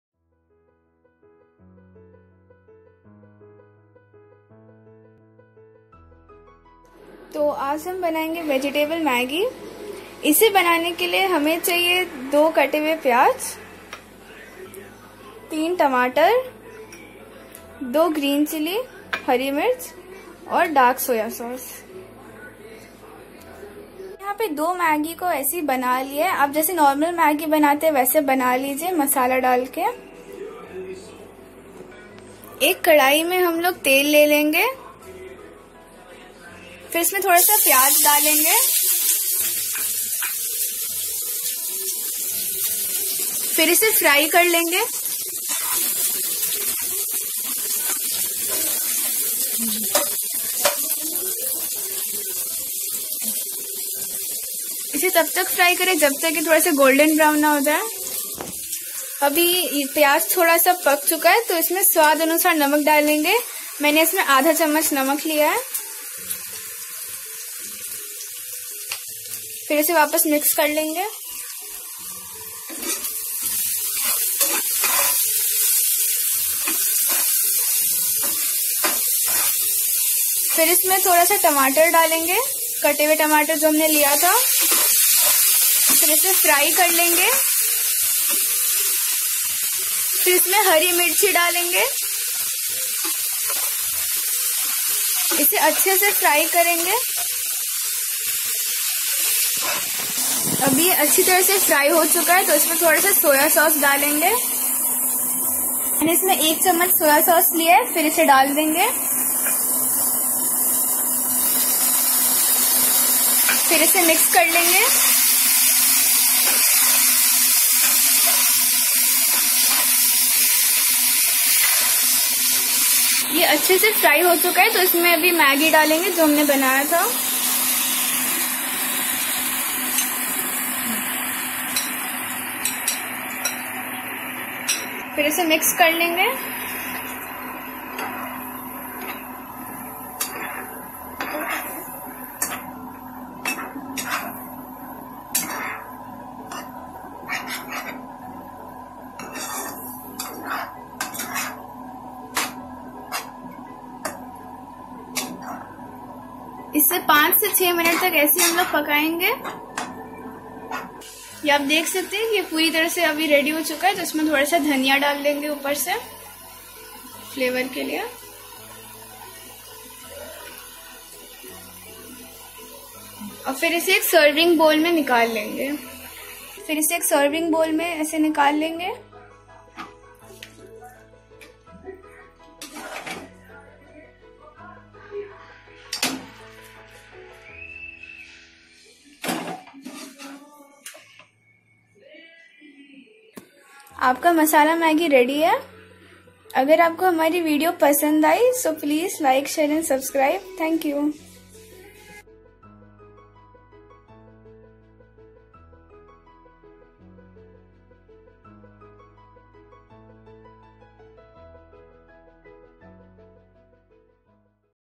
Maggi Masala special Recipe | Maggi banane ki recipe | Vegetable Maggi Recipe
Try out this special and super tasty maggi masala recipe..